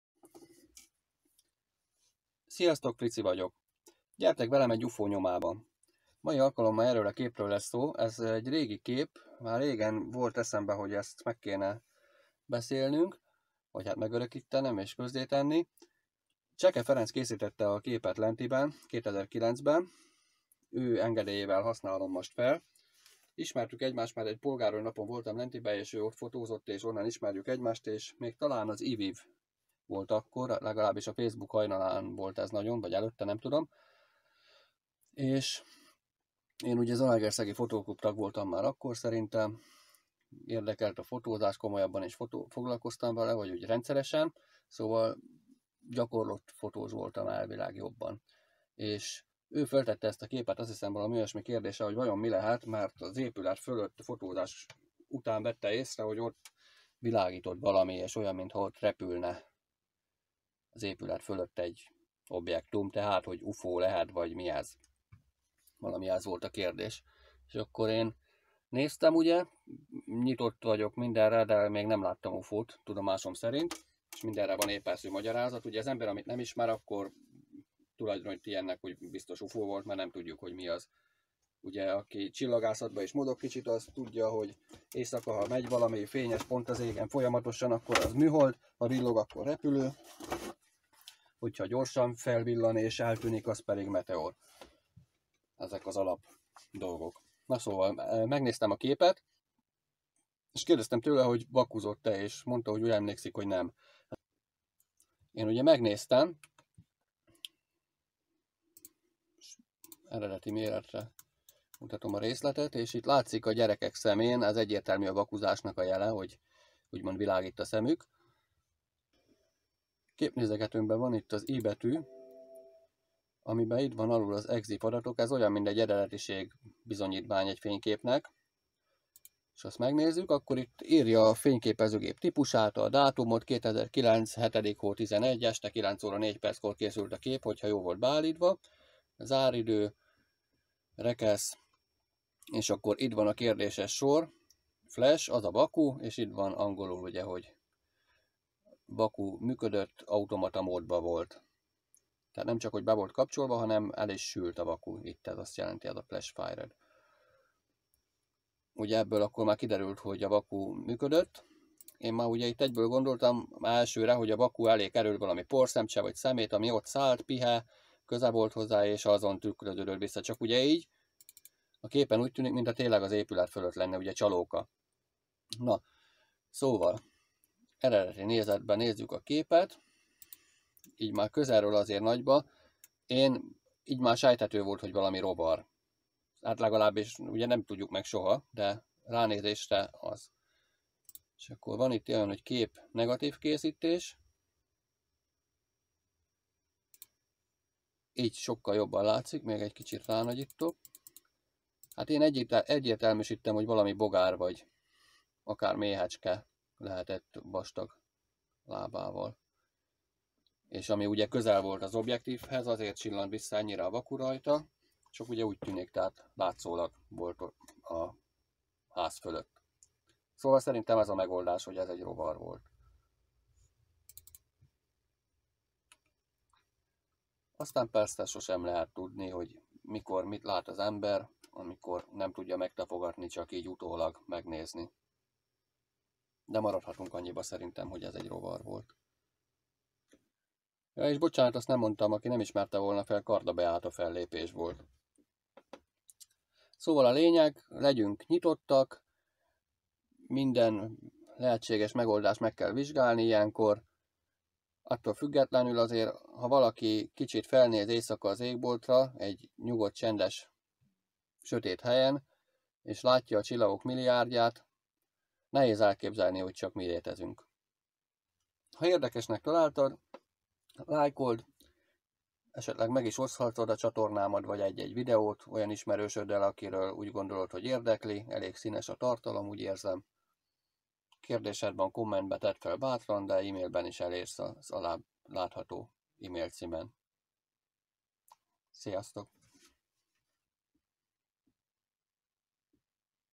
UFO?
Egy UFO nyomába eredünk most. Vajon mi lehetett az a furcsa valami a 2009-ben Karda Beáta fellépésén Lentiben készült képen?
Köszönet a képért Cseke Ferencnek, ő fotózta.